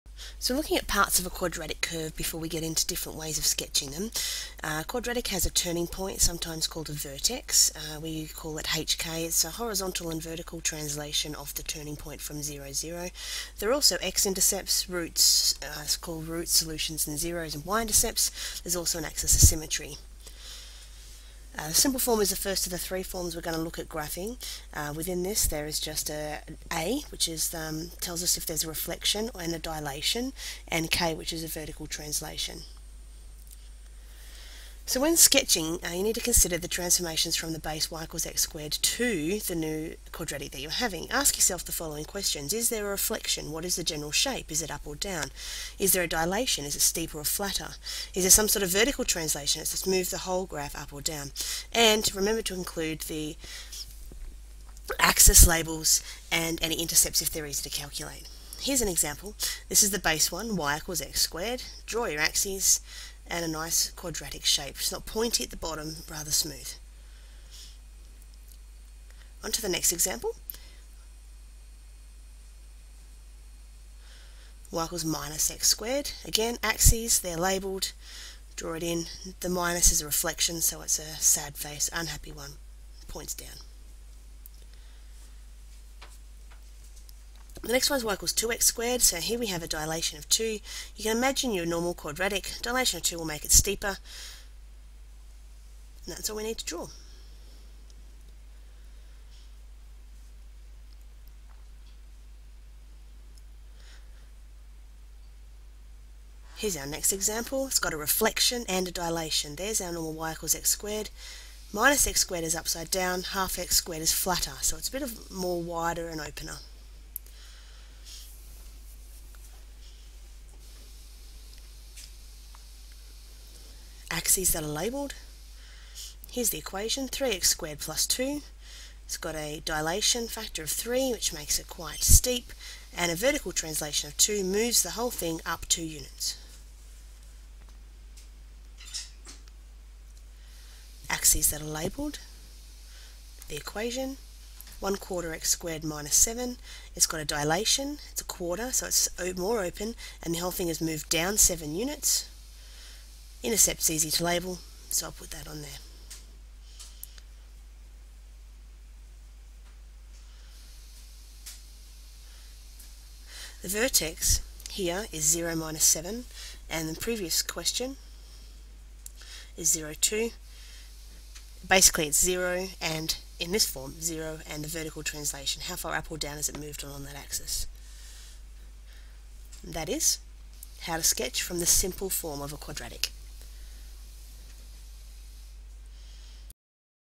Quadratic_1 Simple Form
Series of 4 videos discussing various methods to recognise the features of quadratics and how to sketch them.  Goes through simple form, hk form, fully factorised form and then a summary for the week.  Recorded by Erin Gallagher for the MM1 and SM1 students of Hawker College May 2012.